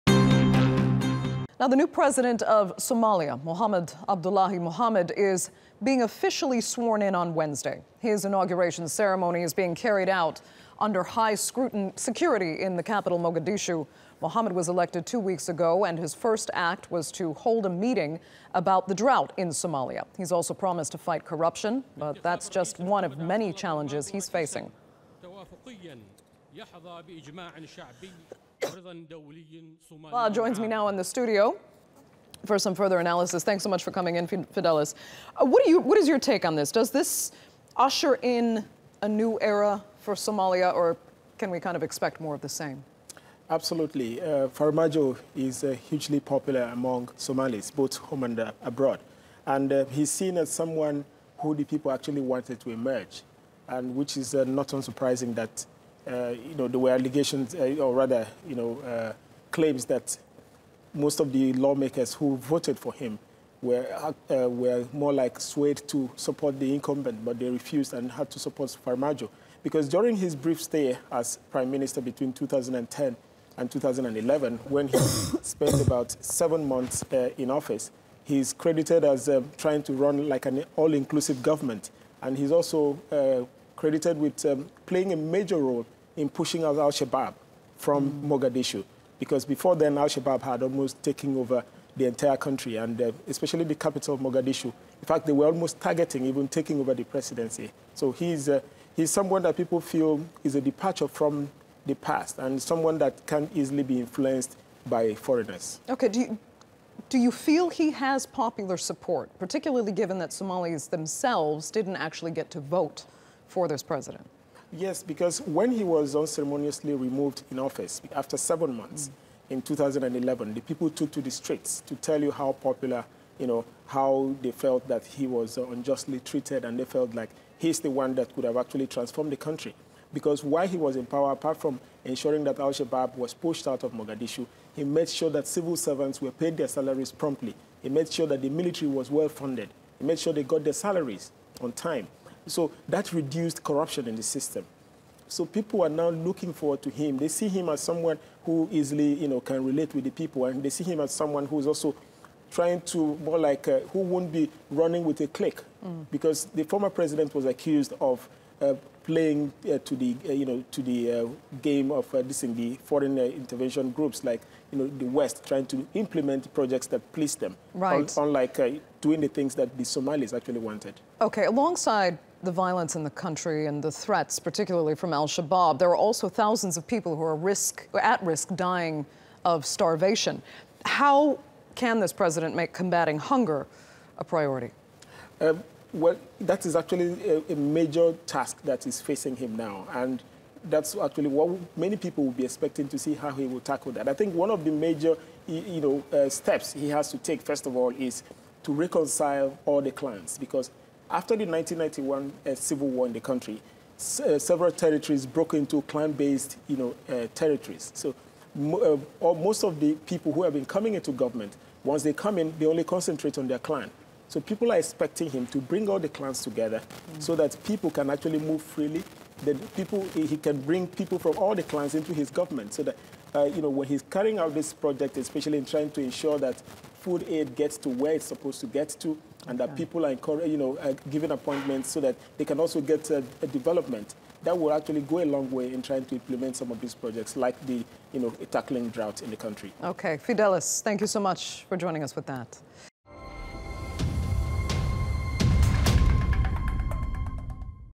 Somalia President Inauguration: Somalia formally appointing new president
The new president of Somalia, Mohamed Abdullahi Mohamed, will be inaugurated on Wednesday in a ceremony performed under high security in the capital Mogadishu. Fidelis Mbah weighs in.